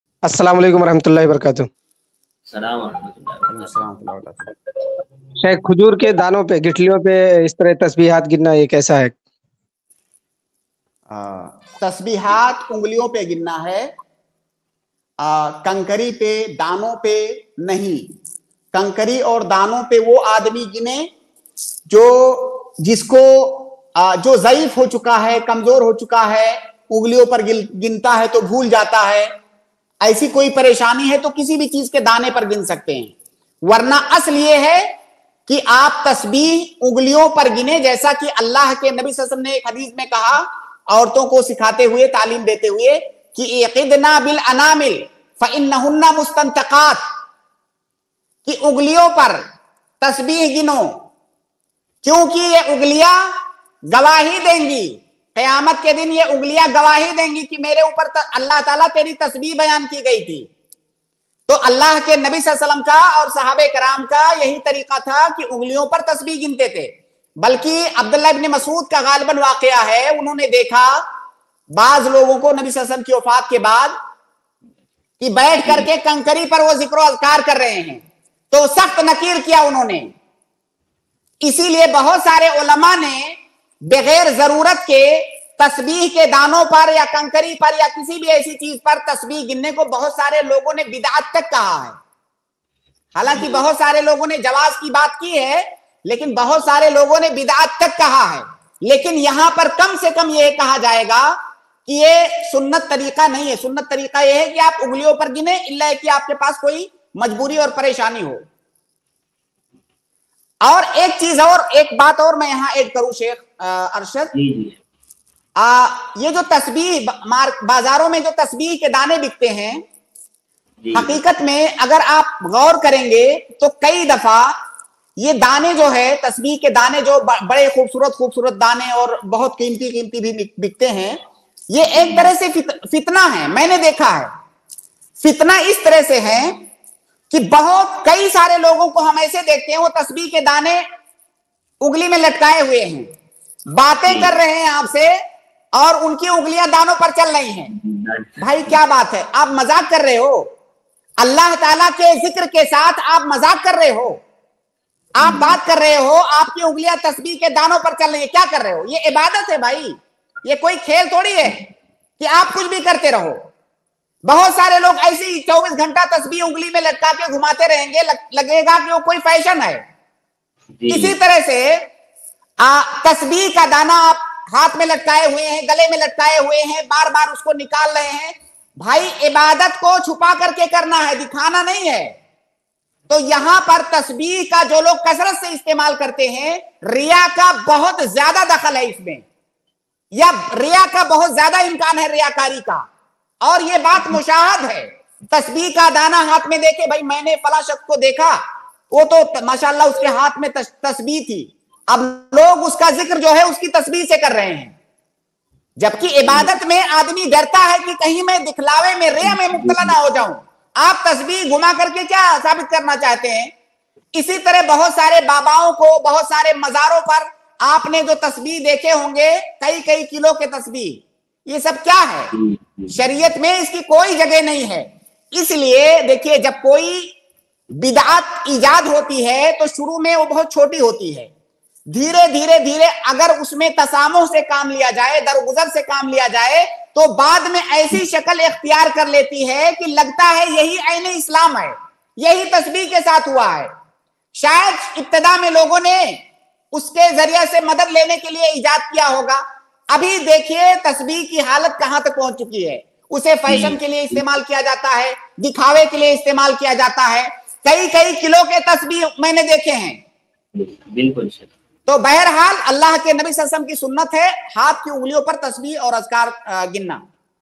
Daano Par Tasbeeh Padhna Kaisa Hai? By Dr. Mohammad Naseem Madani iPlus TV Deeni Q&A
UPI : donateilm@icici